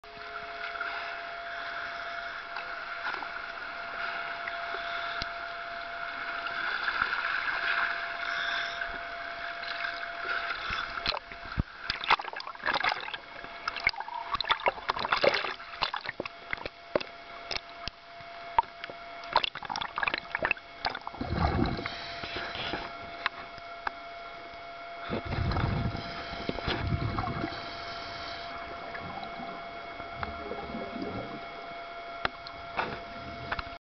Hidden Treasure
This dive was in a deep chasm of a mountain. It goes back several hundred feet to a small cave. Unfortunately, to not get thrown around by the surge I had to hide behind a rock. This meant I couldn't get a very good view of the back of the cut. Eventually I push off and catch up with the rest of my dive buddies.